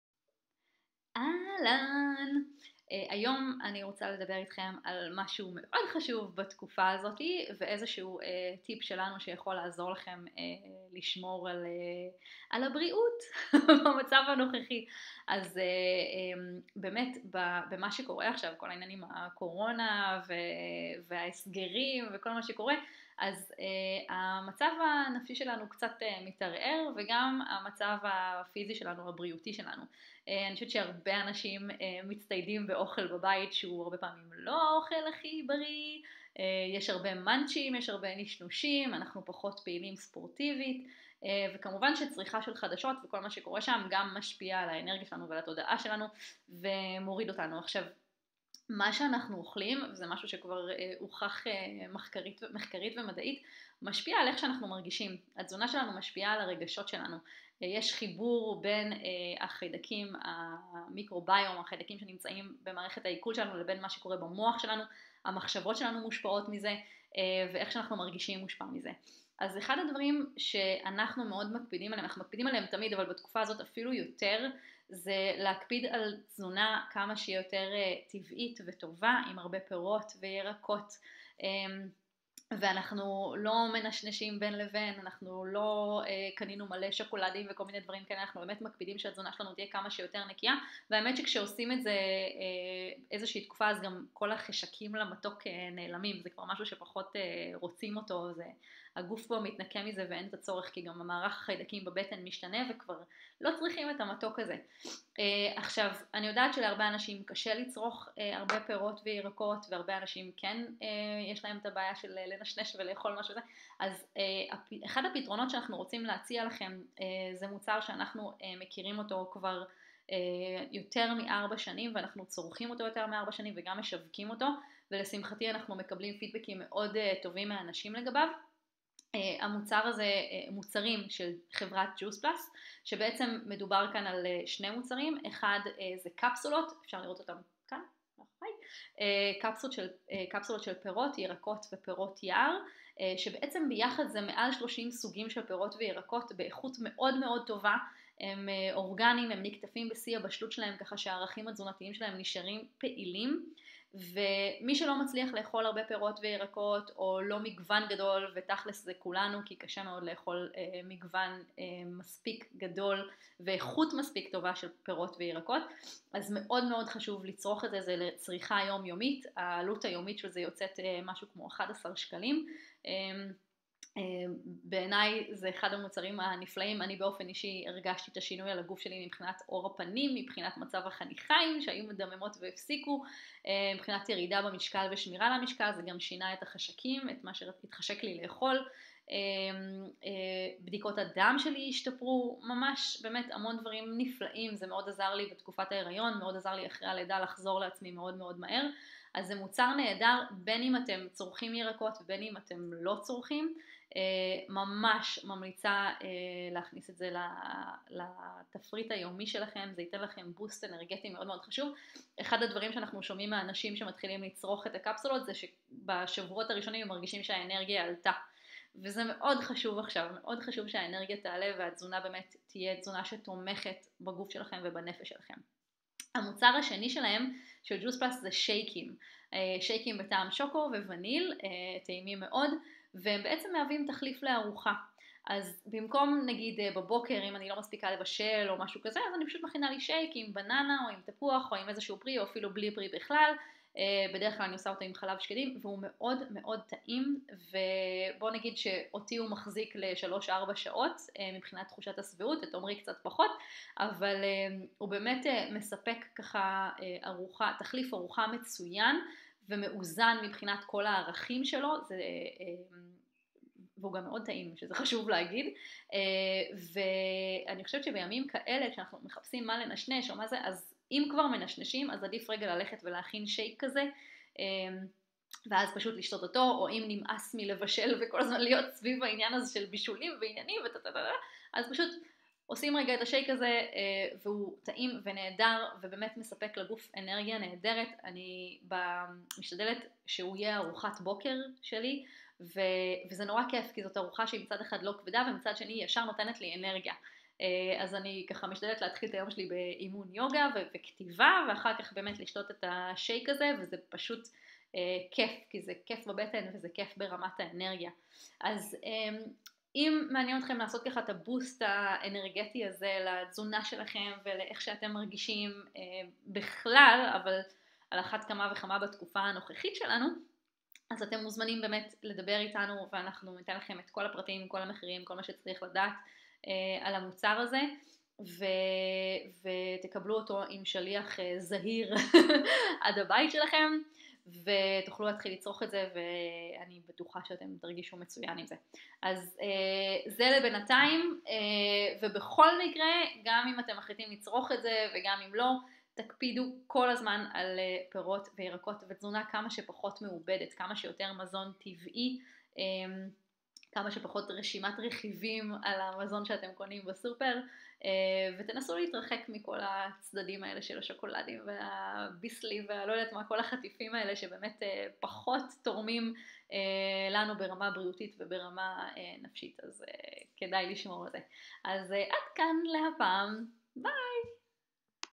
תוספי תזונה חשובים לחיזוק הגוף והמערכת החיסונית בתקופת הקורונה
יש חשיבות אדירה לתזונה שאנחנו בוחרים לתת לגוף שלנו, במיוחד בתקופה של אי וודאות או מלחיצה שבה חשוב מאוד לחזק את המערכת החיסונית ואת מצב הרוח שלנו.
לכן אנחנו ממליצים מאוד על תוספי התזונה האלו של ג׳וס פלאס שמעניקים לגוף מגוון מאוד רחב של ירקות ופירות איכותיים.
אפשר ליצור איתנו קשר דרך
או
הרגלים של מגשימים: